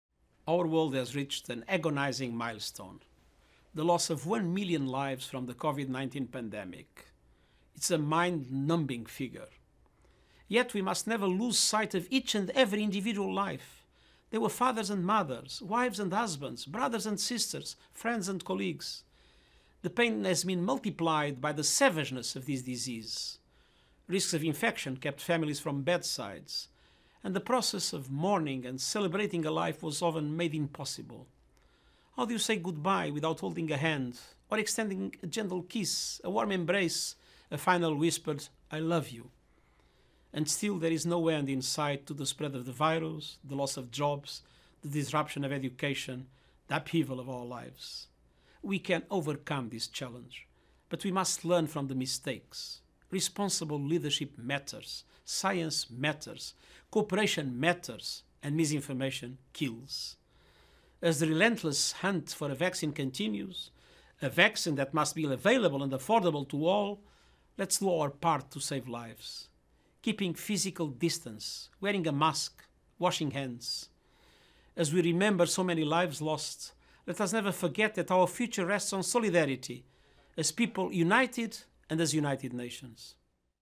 グテレス国連事務総長 コロナ死者100万人でメッセージ
米ジョンズ・ホプキンズ大の集計によると、新型コロナウイルスによる死者数が29日、全世界で100万人を超えた。国連のグテレス事務総長は動画によるメッセージを公表した。（国連提供）2020年9月29日公開